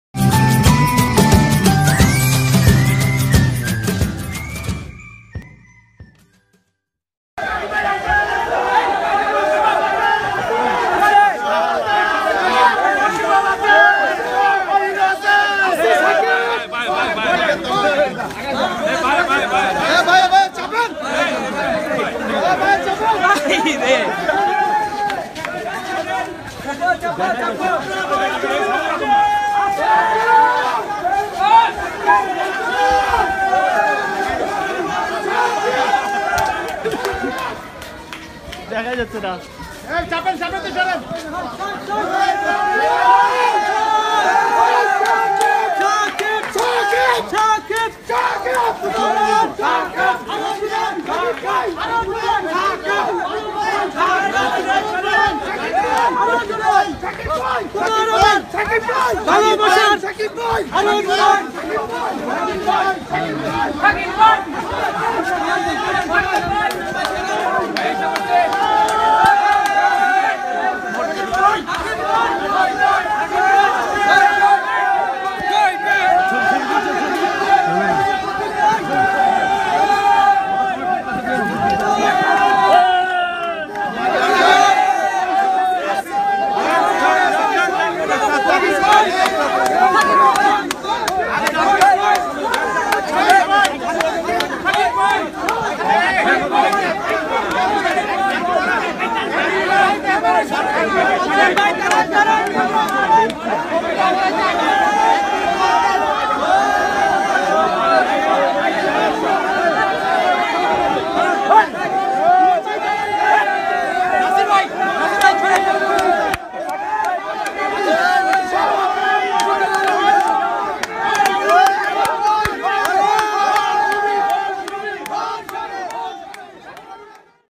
৩টা ২০ মিনিটে ভোট দিতে আসেন সাকিব খান || Funny tv
চলচ্চিত্র সমিতির নির্বাচনে সাকিব খান ভোট দিতে আসে সাকিব খান সময় ৩ টা ২০ মিনিট